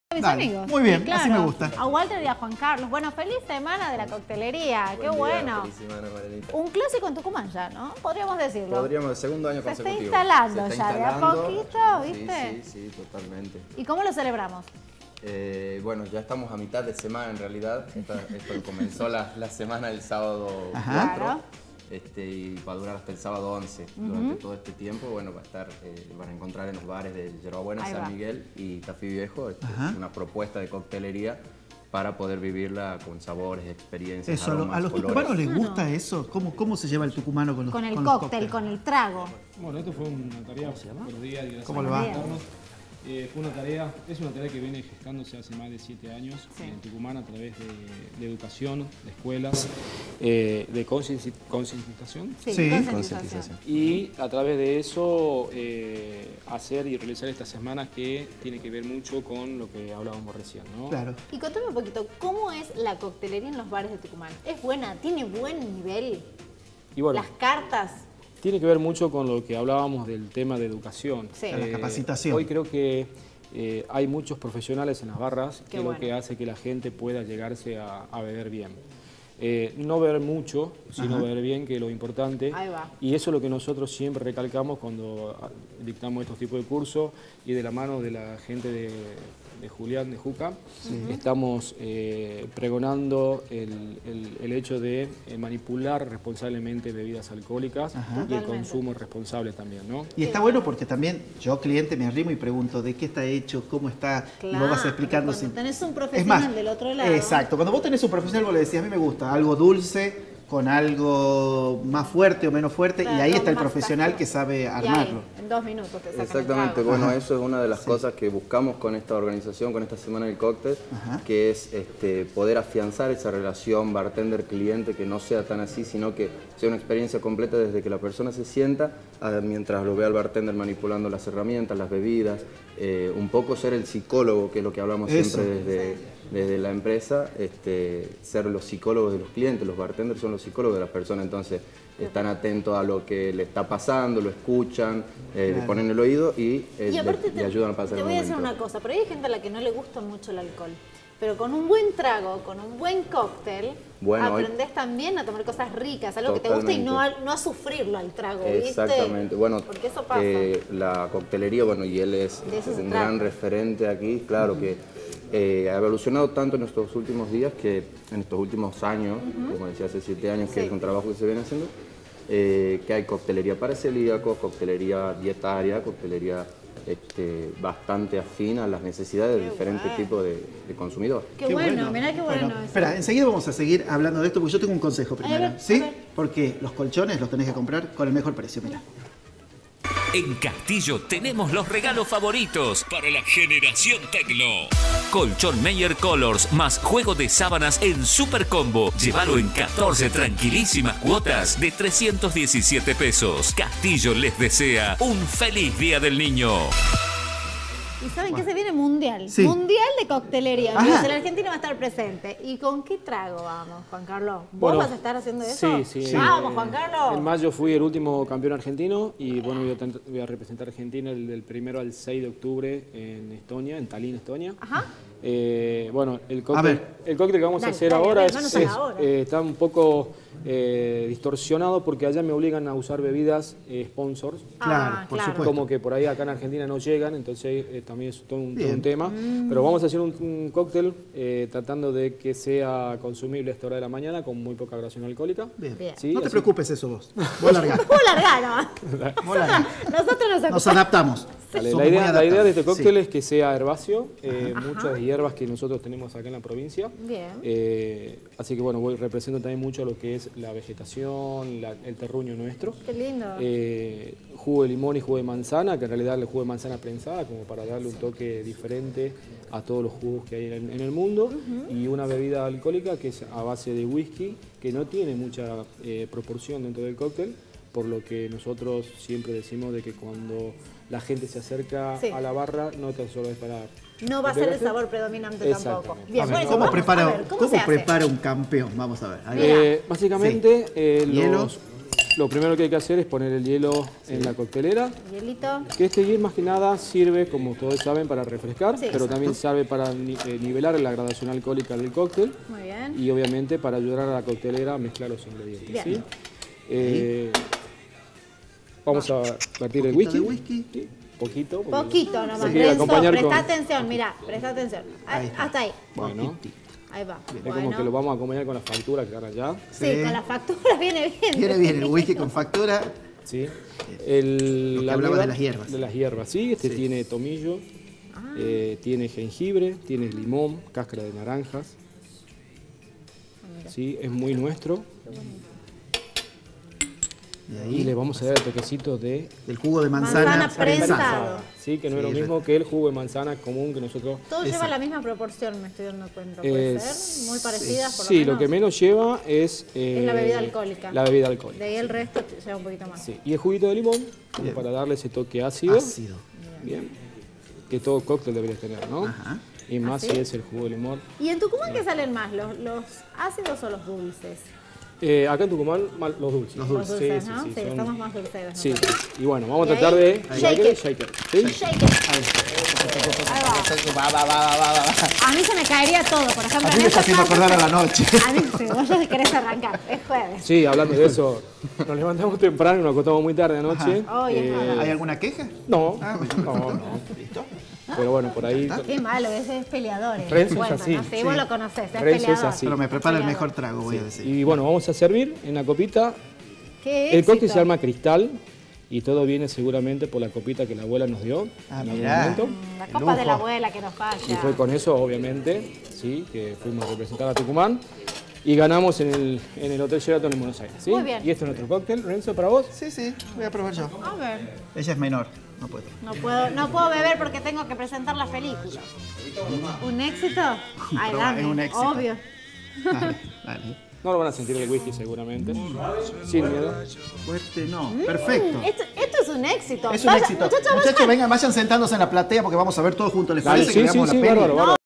Semana de la coctelería: Propuestas en diferentes bares
En el piso de Vivo Tucumán, Julián Capocotta y Walter Sáncehz Loria enseñan hacer un buen trago y cuentan sobre la Semana d ela Coctelería